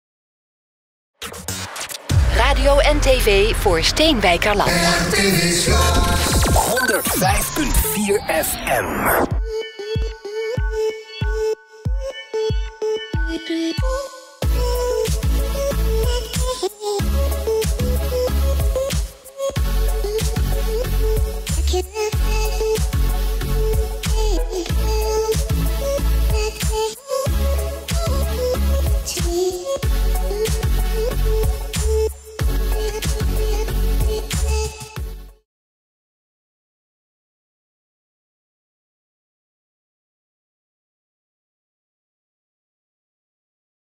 RTV SLOS - Timelapse dag 2 DW
Een timelapse van het uitzicht vanuit de RTV SLOS Studio, tijdens het 29e Dicky Woodstock Popfestival.